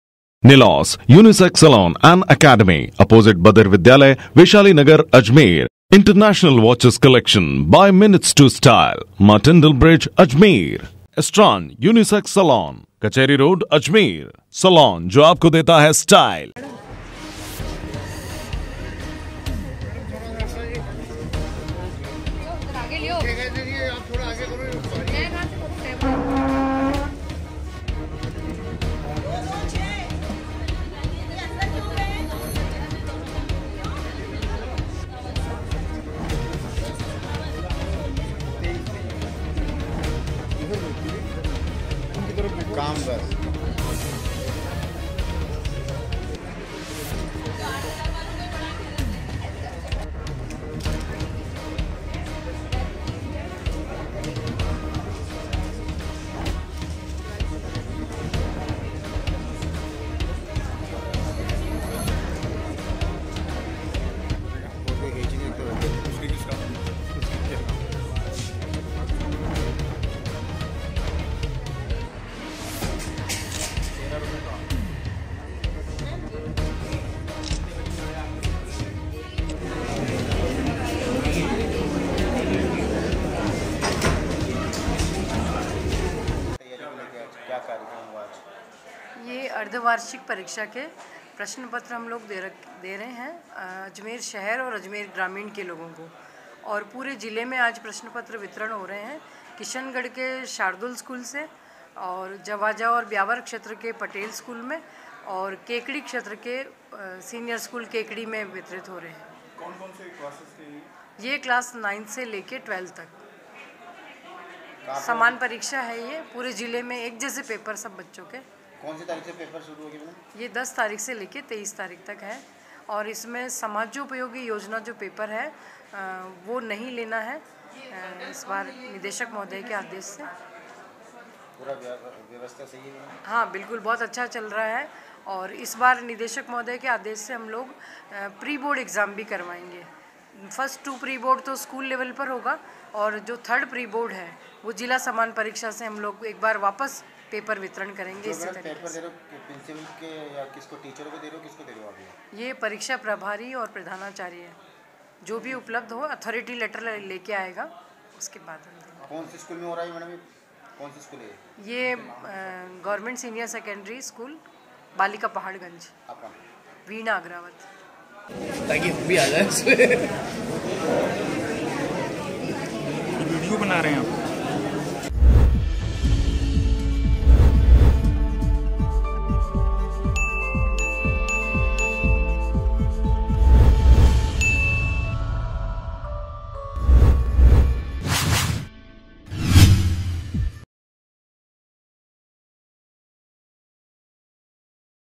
AJMER NEWS | अर्द्धवार्षिक परीक्षाएं 10 दिसंबर से | MTTV INDIA
अर्द्धवार्षिक परीक्षाएं 10 दिसंबर से,जिला समान परीक्षा के अंतर्गत,कक्षा 9 से 12 तक की अर्द्धवार्षिक परीक्षा 10 दिसंबर से 23 दिसंबर तक,आज परीक्षा प्रभारी को पेपर किए गए वितरित